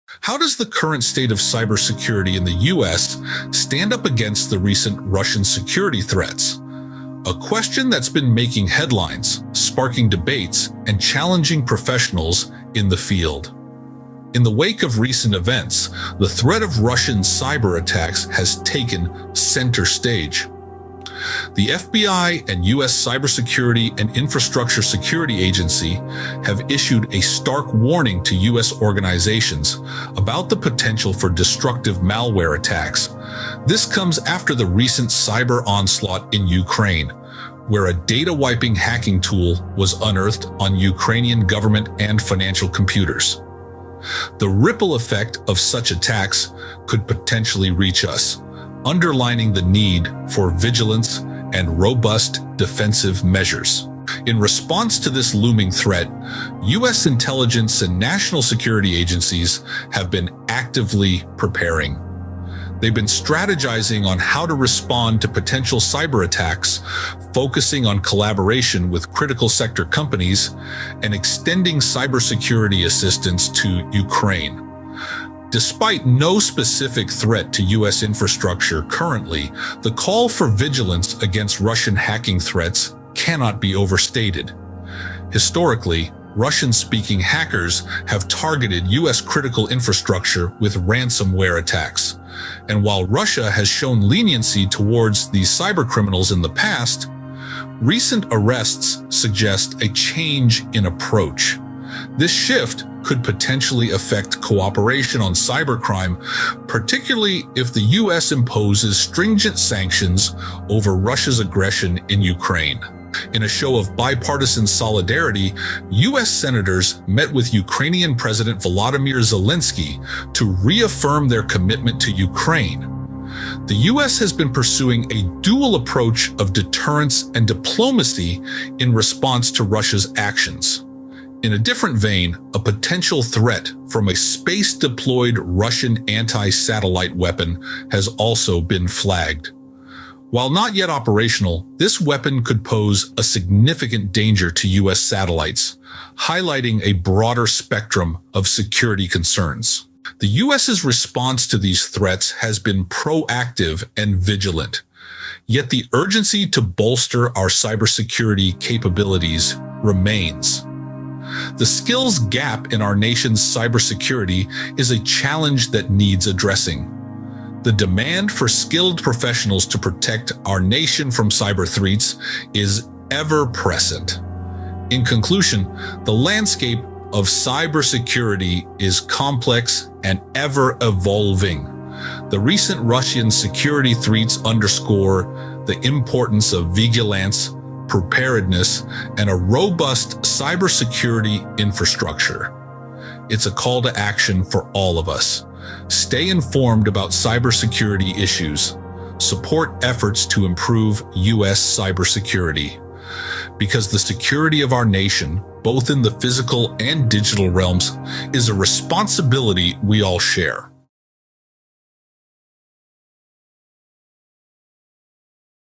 Cybersecurity: The New Frontier in National Security
This content was generated using invideo AI with my voice. I'm starting to learn more about creating video content using AI. Now that I've uploaded my voice profile to the invideo AI platform, I'm testing out different prompts to see how they come across. The content in this video was curated using recent event around Russian anti-satellite technology. While there has not been any credibility to that, it raises awareness on how we as a society can play a role in our nation's cybersecurity.

Let me know what you thought of this AI generated content. While the video was generated by AI, this description was not.